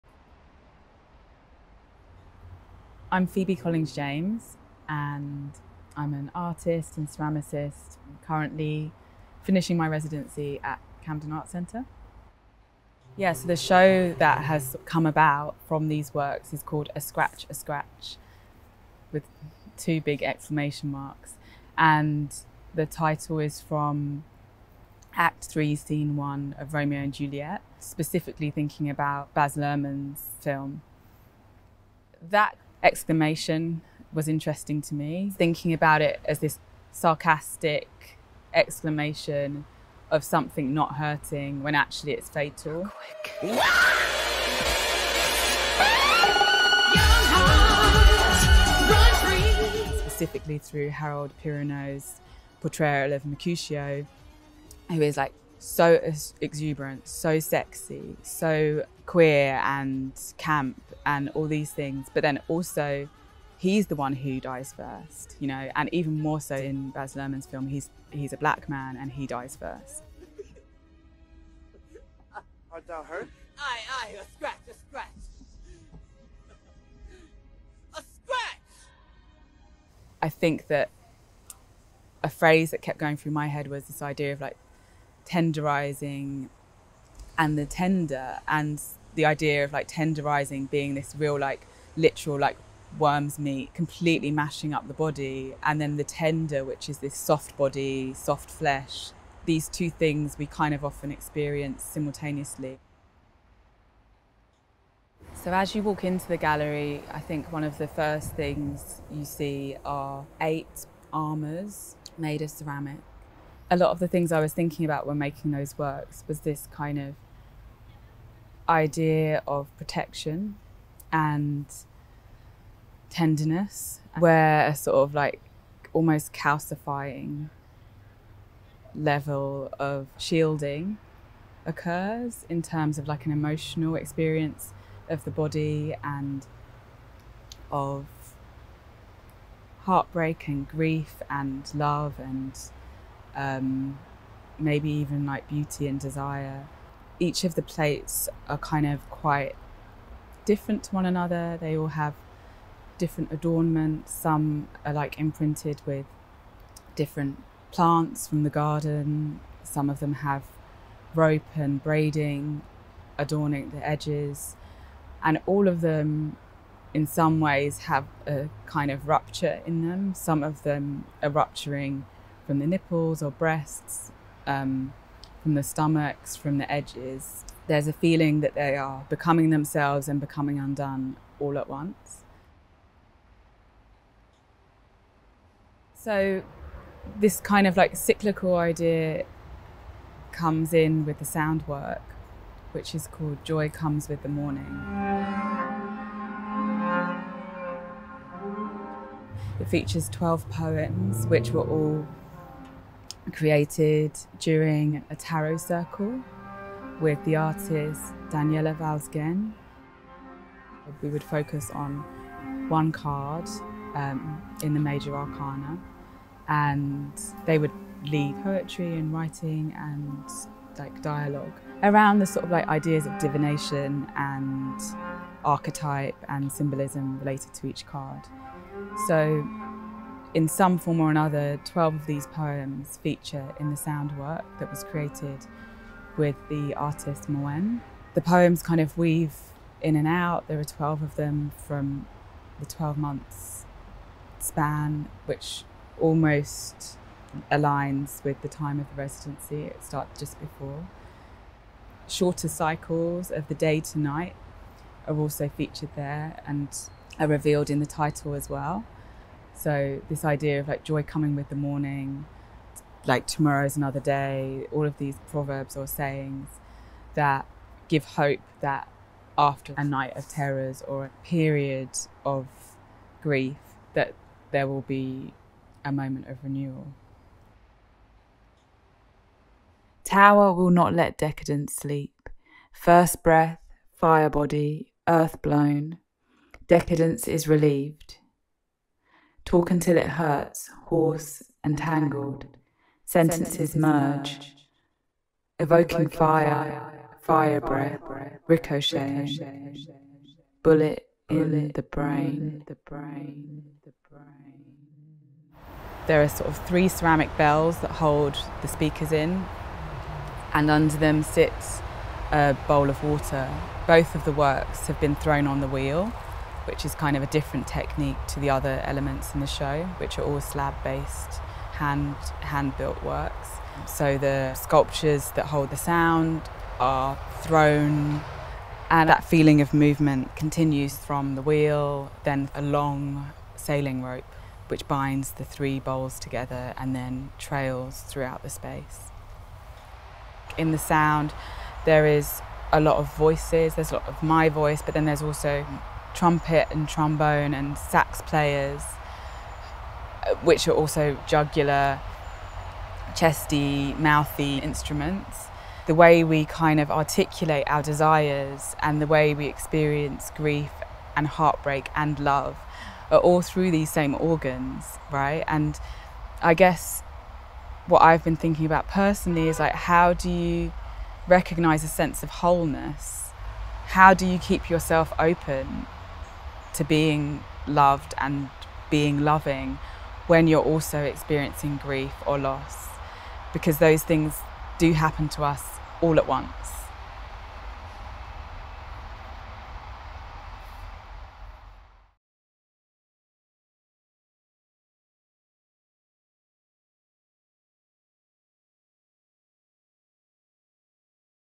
Phoebe Collings James on A Scratch! A Scratch! at Camden Art Centre, 2021
Phoebe Collings-James’ wide-ranging practice encompasses sculpture, video, sound and performance. For her first solo exhibition in a UK institution, she has created a sensual environment combining sound and sculpture that unfolds from undercurrents of grief, heartbreak and desire.

This film was produced by Jared Schiller for Camden Art Centre on the occasion of Phoebe Collings James: A Scratch! A Scratch!, 10 September - 23 December 2021.